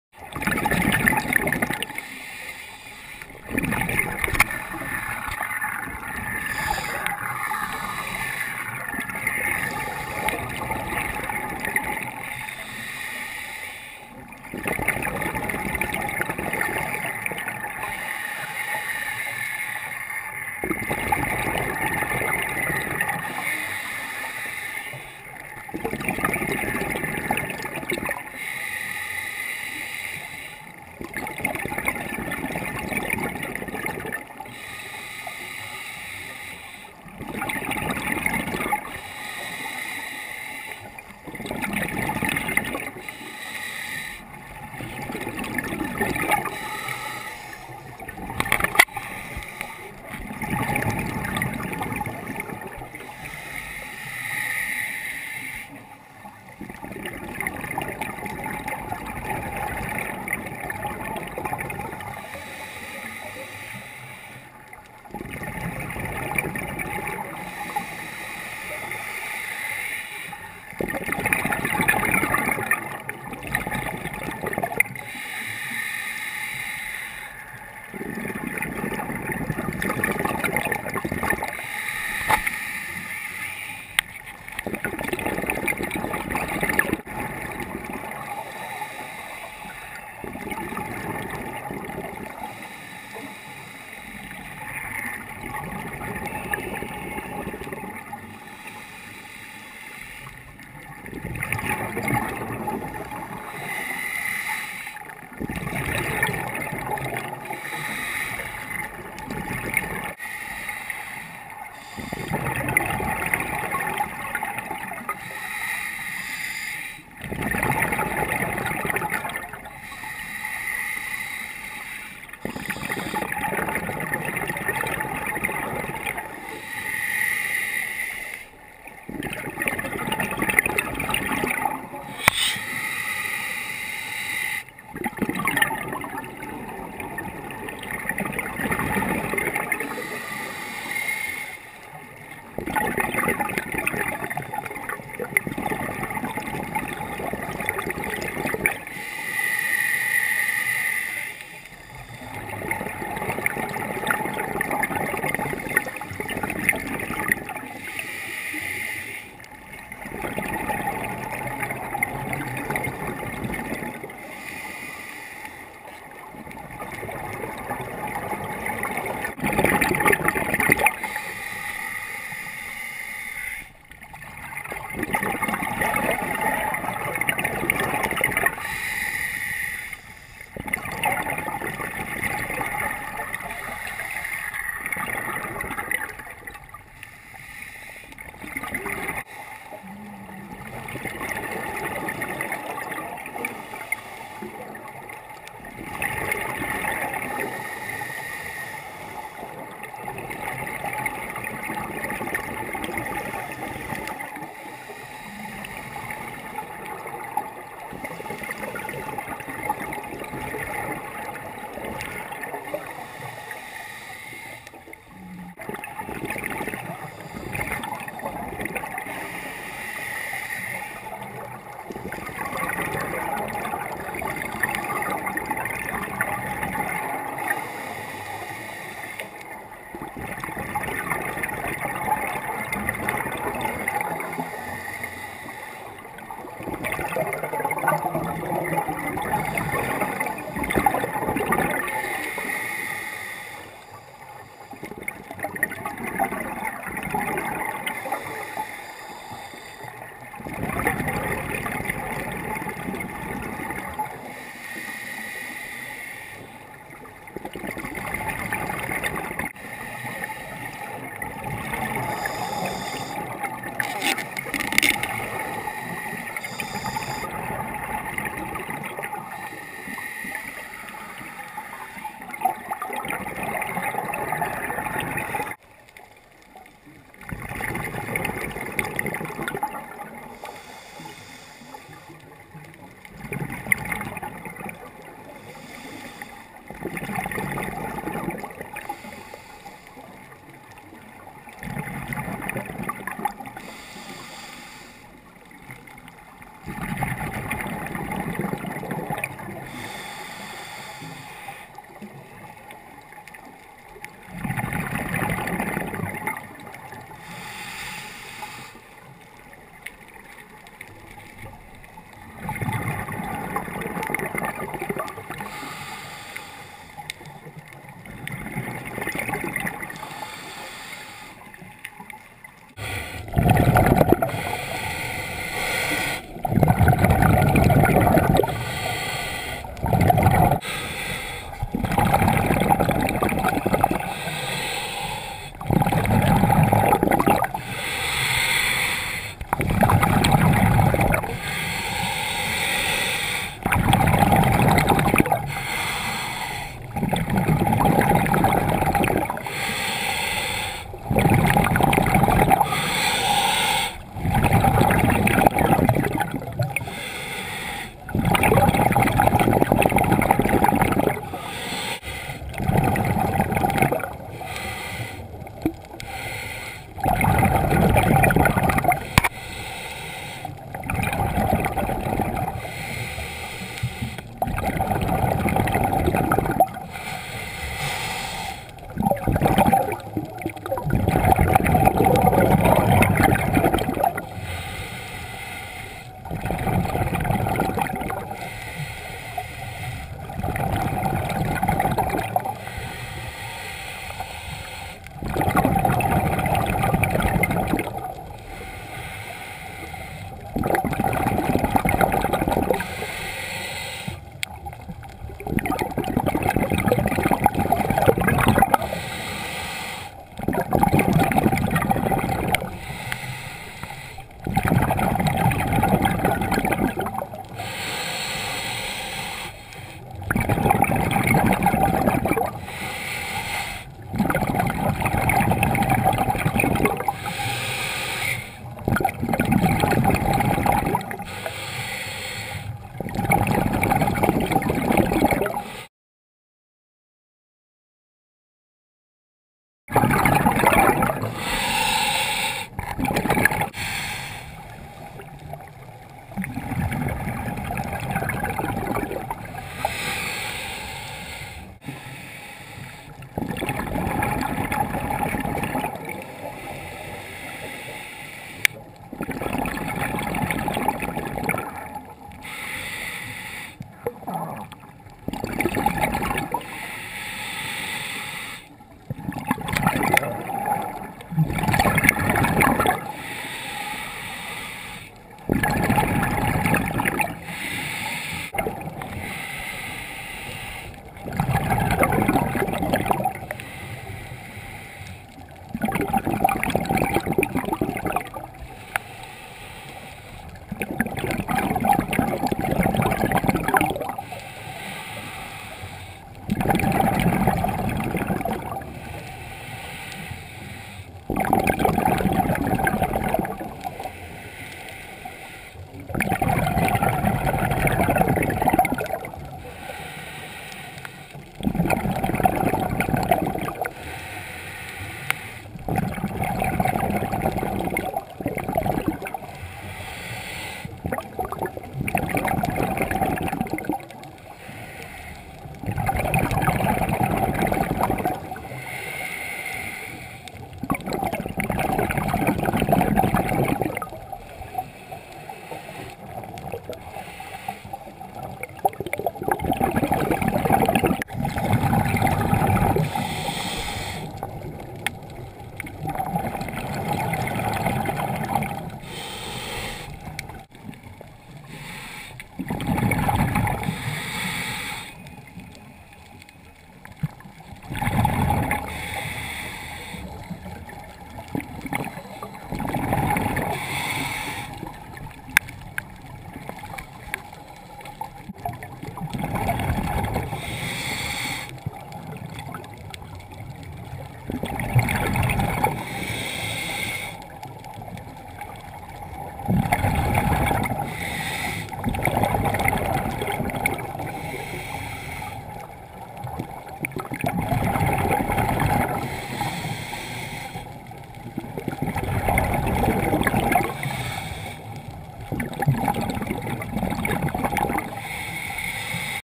diving in cuba pt 2
Diving in Varadero Cuba, About 12 meters depth. Go Pro HD Hero camera used. Got a little bit foggy but otherwise fine.